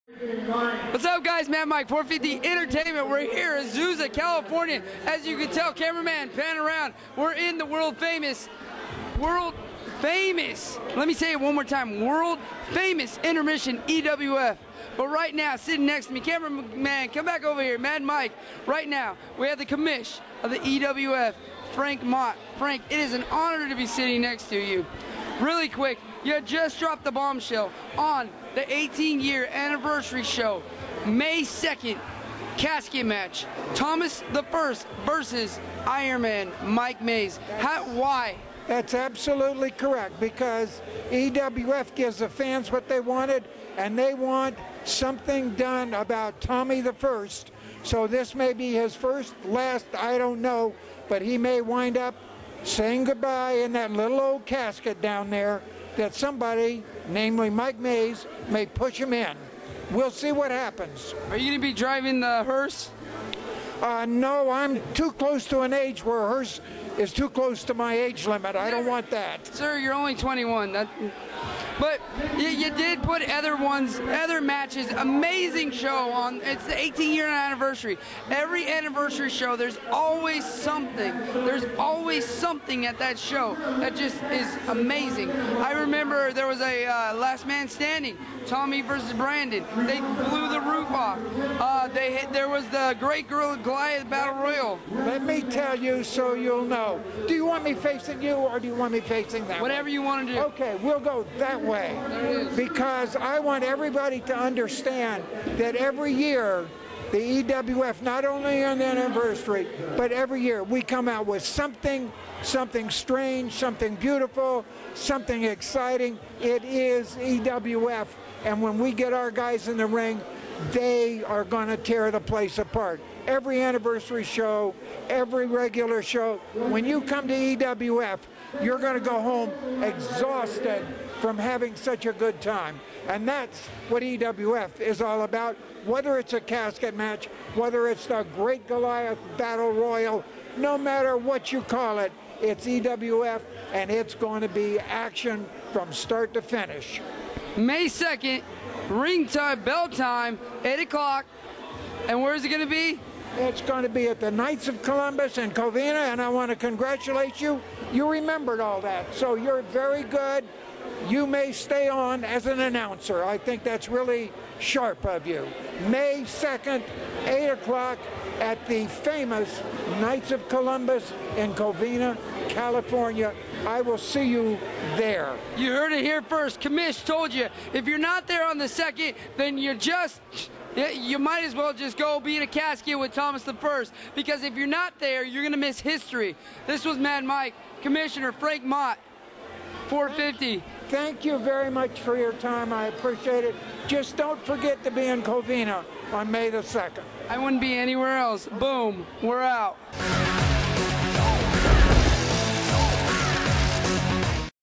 (450 ENTERTAINMENT) Interview EWF Commissioner Frank Mott
450 ENTERTAINMENT caught up with EWF Commissioner FRANK MOTT as he explains exactly what the EWF brings to the fans each and every event!